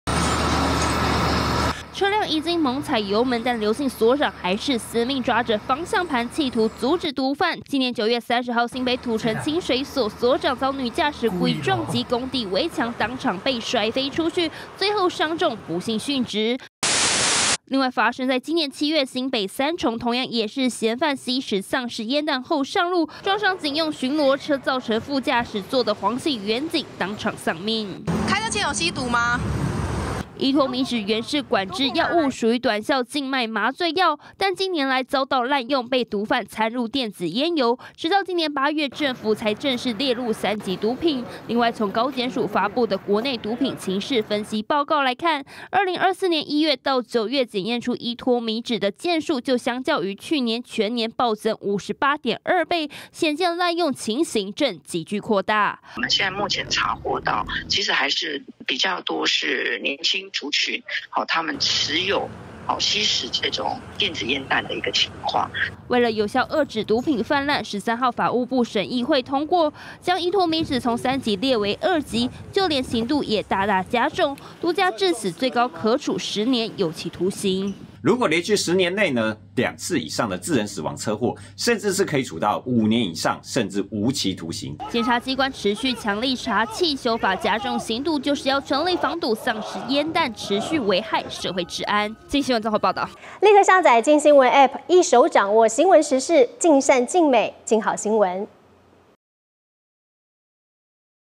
20241116鏡新聞防「喪屍煙彈」猖獗！ 「依托咪酯」改列二級毒品｜鏡新聞
『立勤小學堂』
喪屍煙彈毒害國人甚深，特別是吸食後又駕駛交通工具，常常造成更多傷亡，因此，也被列為『毒駕』的一種毒品，吸食後駕駛，就可以處一年以上三年以下有期徒刑，如果因此致人死亡，可以處到三年以上十年以下有期徒刑，如果十年內發生兩次以上，還可以處到無期徒刑或五年以上有期徒刑。
新聞網址：
『鏡新聞』防「喪屍煙彈」猖獗！　「依托咪酯」改列二級毒品
感謝記者張景涵採訪報導
俗稱「喪屍煙彈」的新興毒品「依托咪酯」，近期在國內大量流竄，甚至造成多起毒駕及重大死傷事故，對此法務部「毒品審議委員會」，昨天（13日）開會通過，「依托咪酯」由3級毒品改為2級毒品，就連刑度也提高，若毒駕致死罪，最高可處10年有期徒刑，累犯者則是無期徒刑。